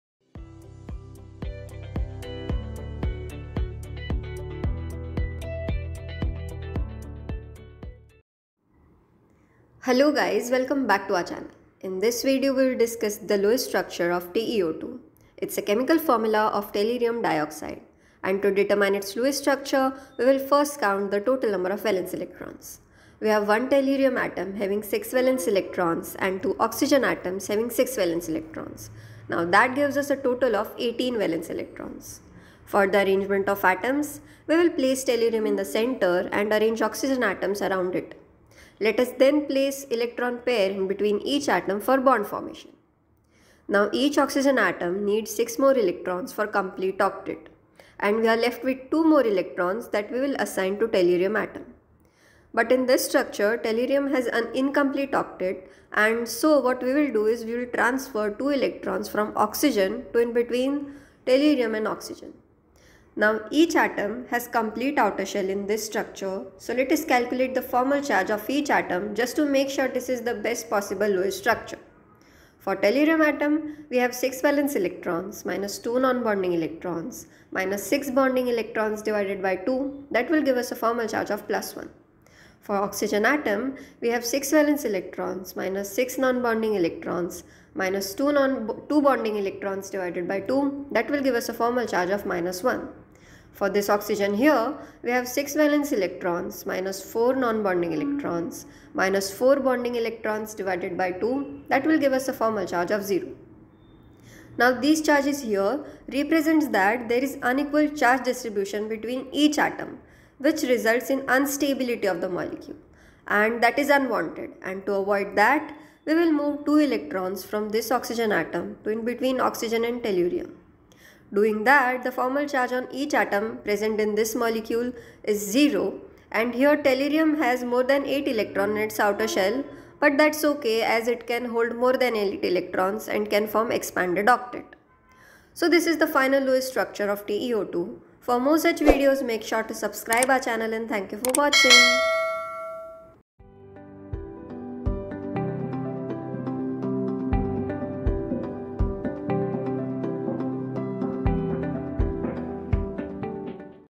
TeO2 Lewis structure: How to Draw the Lewis Structure for TeO2 (Tellurium dioxide)
TeO2  is a chemical formula for tellurium dioxide. And to help you understand the Lewis Structure of this molecule, we are going to share our step-by-step method using which one can find out the Lewis structure of any given molecule.

TeO2 consists of one tellurium atom and teo oxyegn atoms . Watch this video to find out its Lewis Structure with ease.